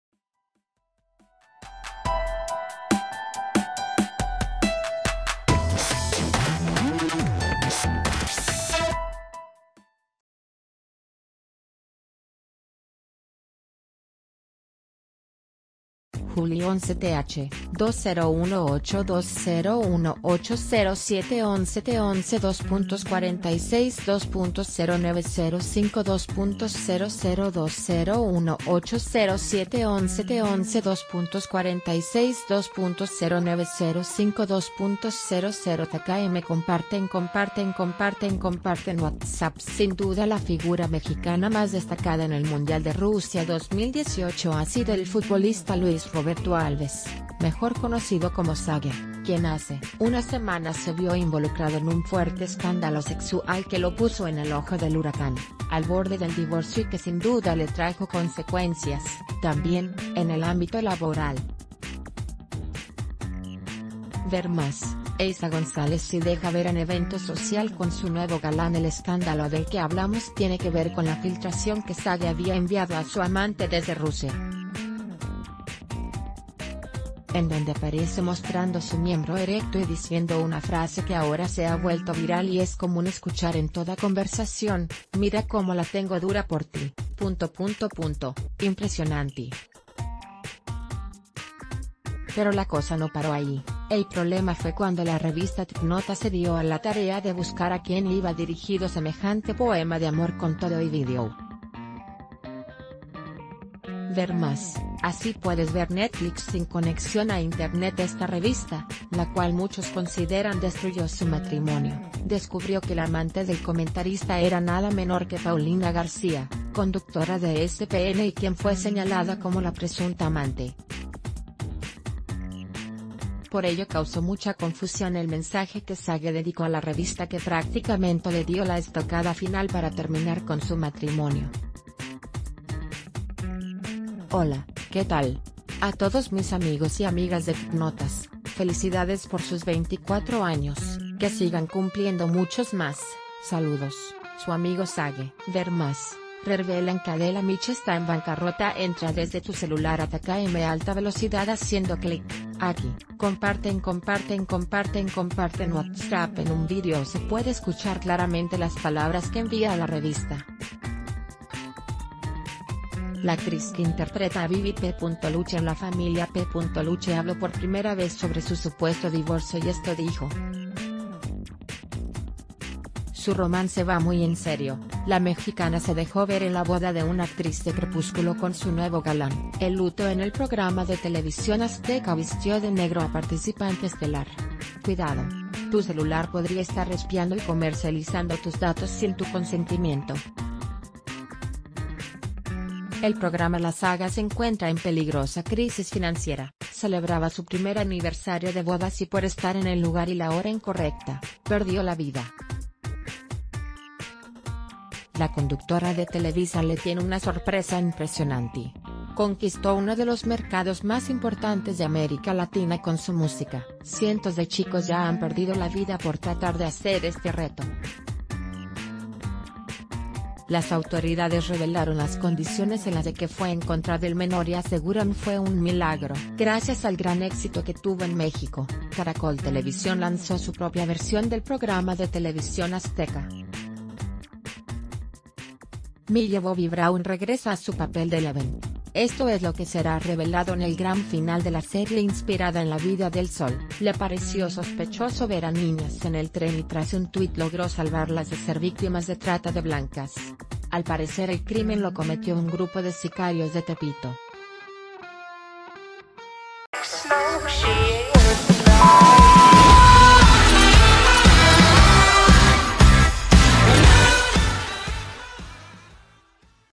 Zague manda mensaje a la revista que destruyó su matrimonio
July 11th, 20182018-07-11T11:46:09-05:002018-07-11T11:46:09-05:00TKM Comparte enComparte enComparte enComparte en Whatsapp Sin duda la figura mexicana más destacada en el Mundial de Rusia 2018 ha sido el futbolista Luis Roberto Alves, mejor conocido como Zague, quien hace unas semanas se vio involuc...